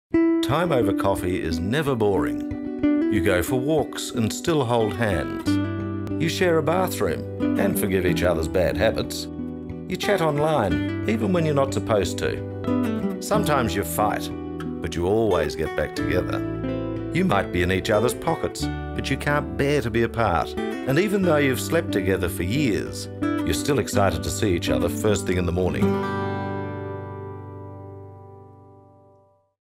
A lot like love - The Mercury
Feel the Love in The Mercury's new TV ad.  Created and produced entirely in Tasmania.  Music composed and recorded by Cary Lewincamp  at Digital City Studios in Cygnet, Tasmania.